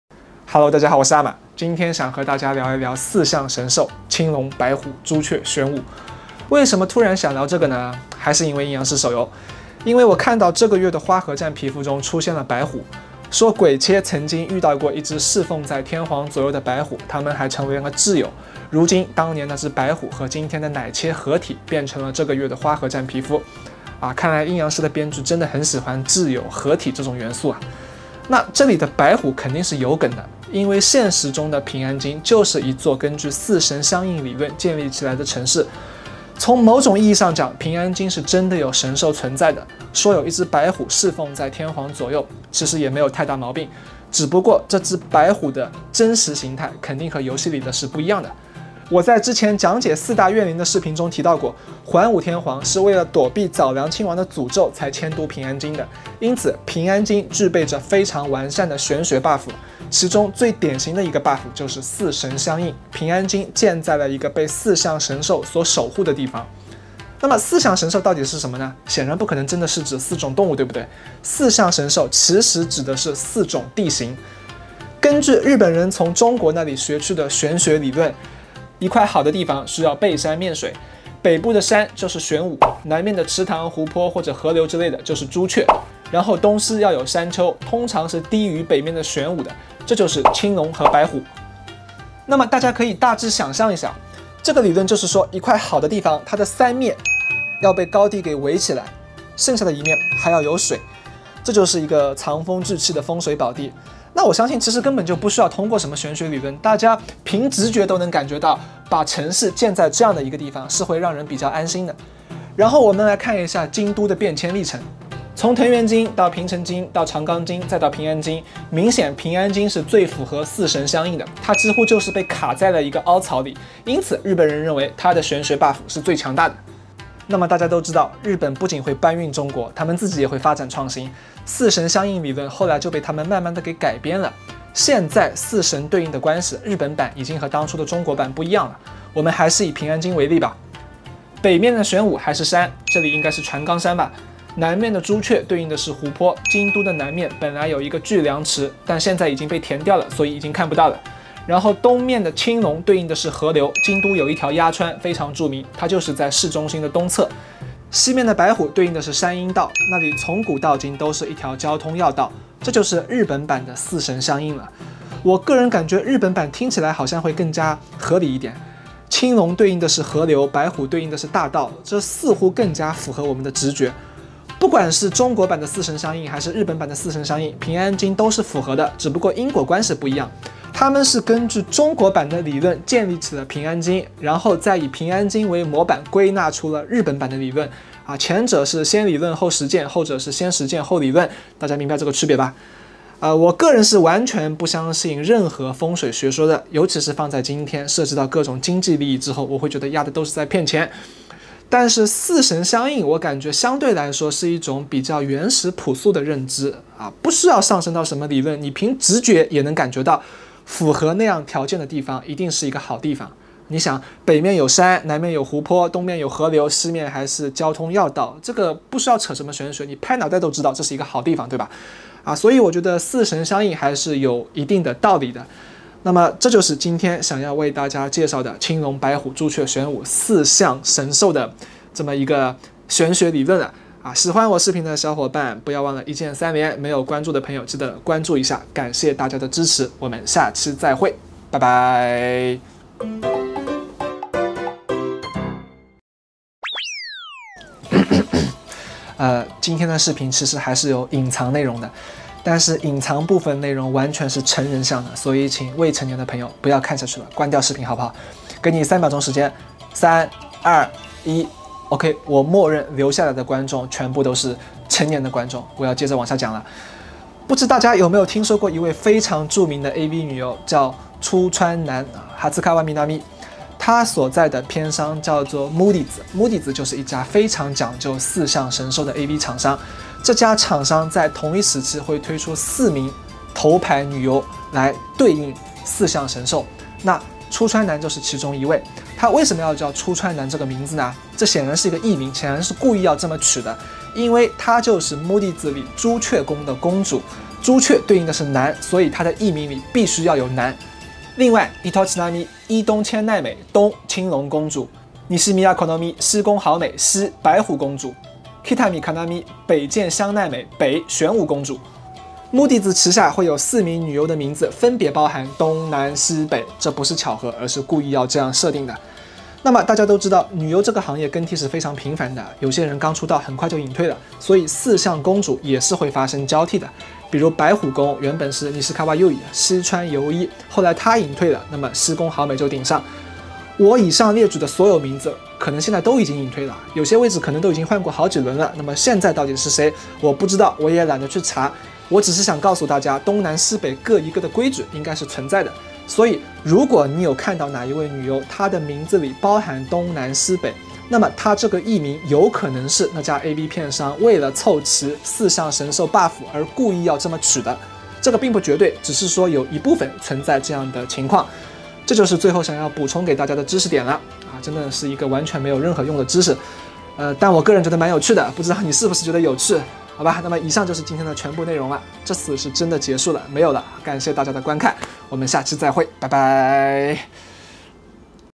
玄學科普！平安京的選址有何玄機？四象神獸的本體是啥？
陰陽師崽戰新皮，風花雪月夜：花信風、狐之嫁、雪化粧、十六夜、夏夕空到底是啥意思